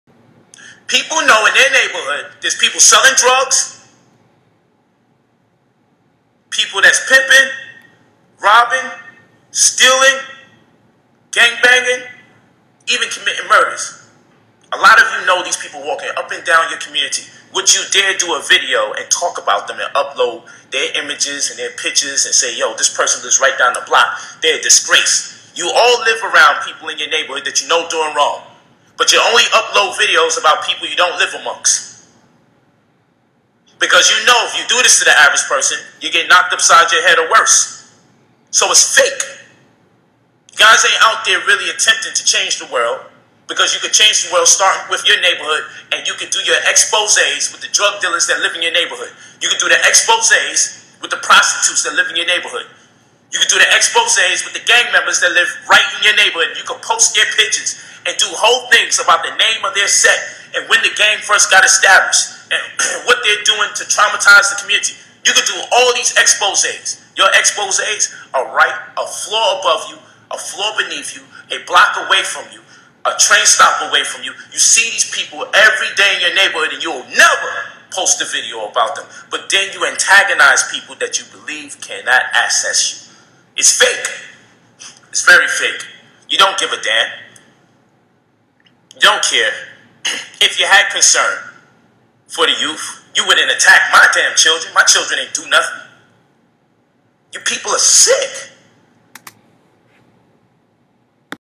BROTHER POLIGHT Shocking Confession About Alleged Rape Allegations.
Real Spit !